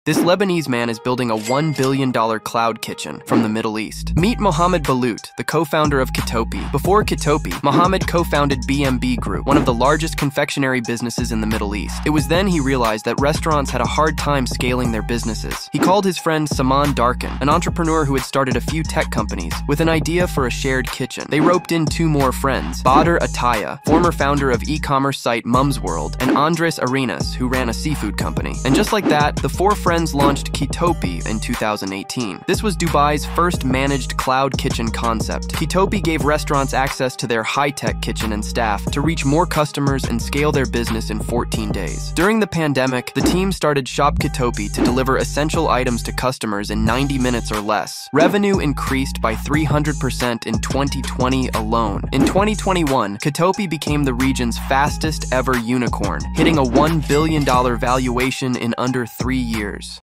$1 Billion Under 3 years: Kitopi Or The Middle East’s Fastest Unicorn 🍱🦄
Follow @thirdnetwork ⤵️